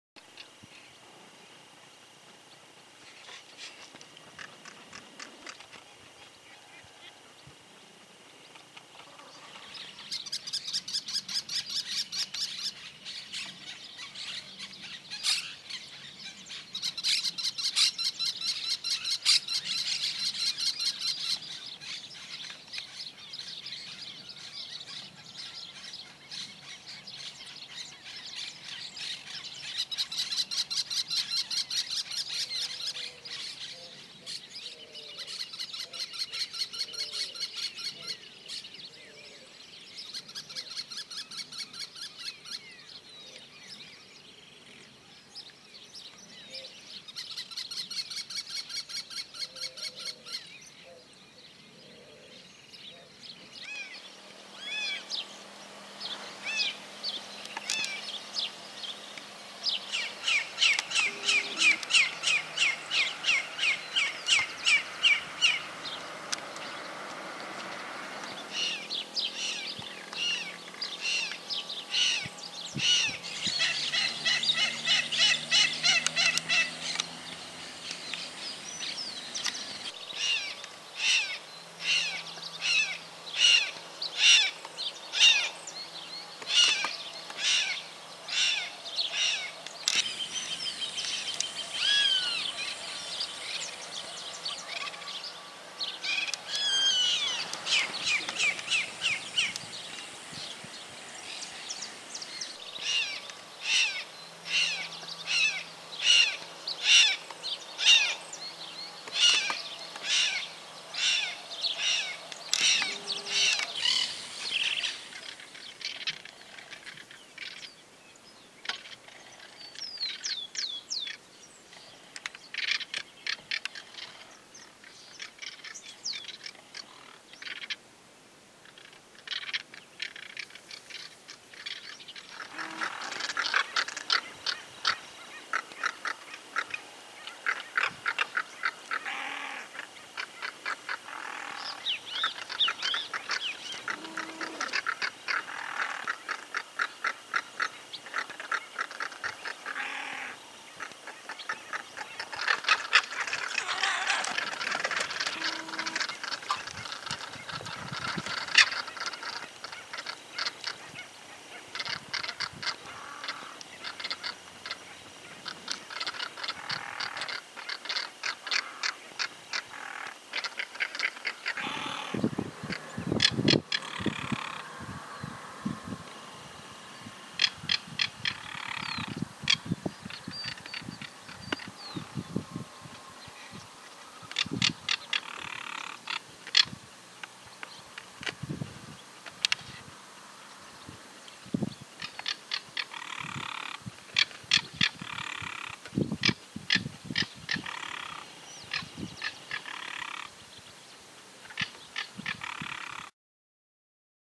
Cantos y gritos de las aves 11º Parte Santa Fe Argentina 2019
Carancho, Caracara plancus, Chimango, Milvago chimango